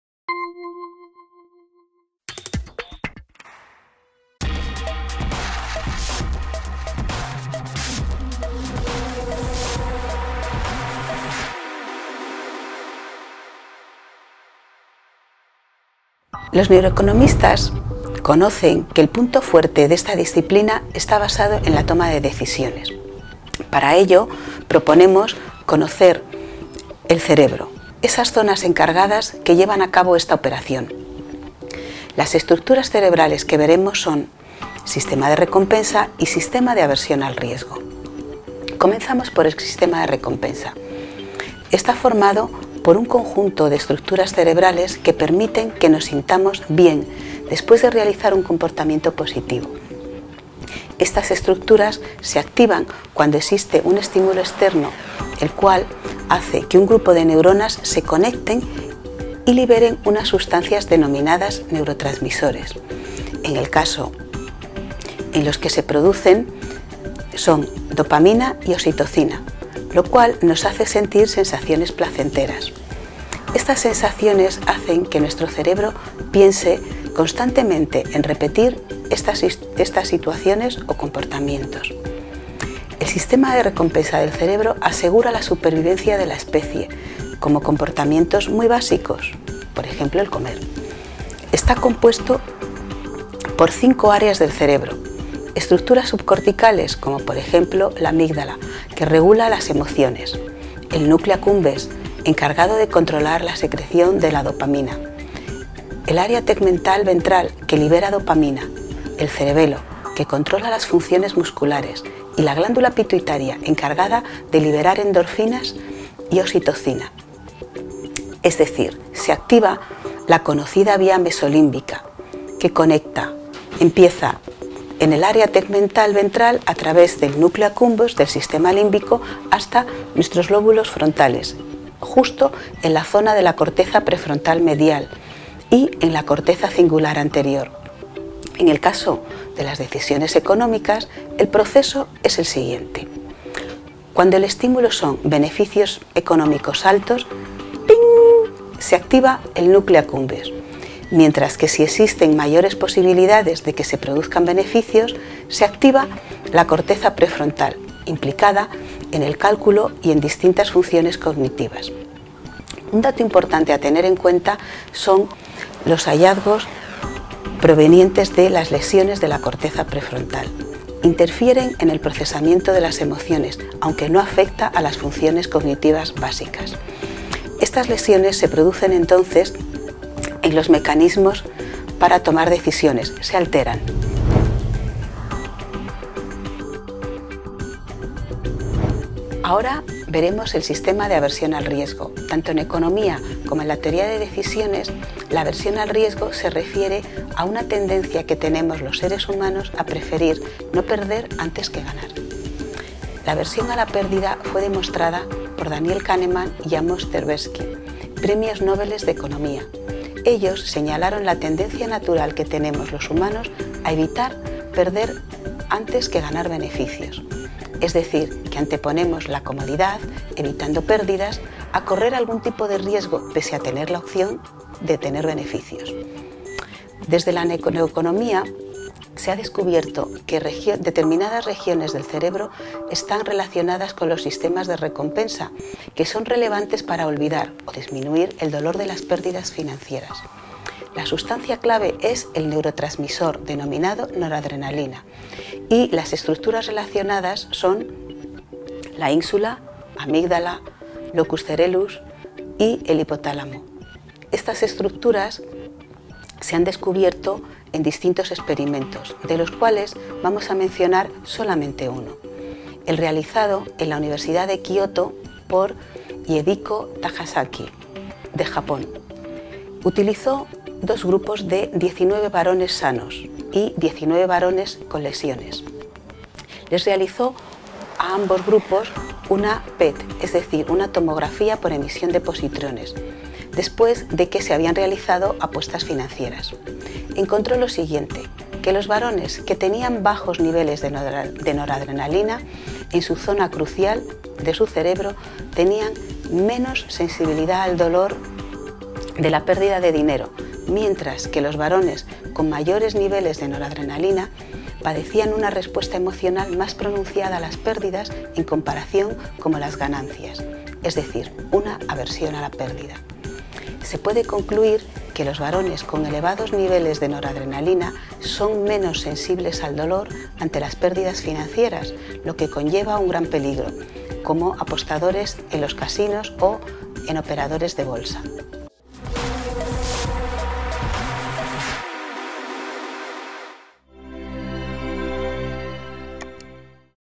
MOOC NEUROCIENCIA. Toma de decisiones: entre el sistema de recompensa y aversión al riesgo
URJCx-MOOC Neurociencia Empresarial: Siete Claves para la PYME - MOOC NEUROCIENCIA. Toma de decisiones: entre el sistema de recompensa y aversión al riesgo
Módulo 5: Neuroeconomía
Conocer el cerebro para la toma de decisiones: sistema de recompensa y sistema de aversión al riesgo.